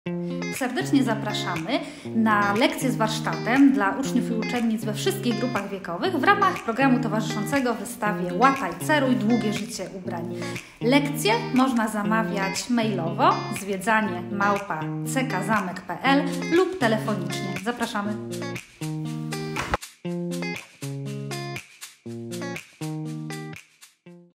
Lekcje dla szkół na wystawie "Łataj! Ceruj! Długie życie ubrań"
Lekcja z warsztatem
W czasie 1,5-godzinnej lekcji poznamy rolę przekazów medialnych, reklamy i social mediów w budowaniu obrazu przemysłu odzieżowego. Przyjrzymy się krytycznie otaczającym nas treściom i zastanowimy się, jak je filtrować i weryfikować.

Drugi wariant lekcji ma aspekt praktyczny: spróbujemy szycia i łatania, by naprawić odzież i ją upiększyć.

Doskonałe uzupełnienie lekcji plastyki, wychowawczej, historii i teraźniejszości czy edukacji wczesnoszkolnej!

Lekcje odbywają się od poniedziałku do piątku w g. 9-12
Koszt lekcji z warsztatami: 10 zł za osobę + 150 zł opłaty za lekcję
Opiekunowie(-ki) uczestniczą w zajęciach bezpłatnie.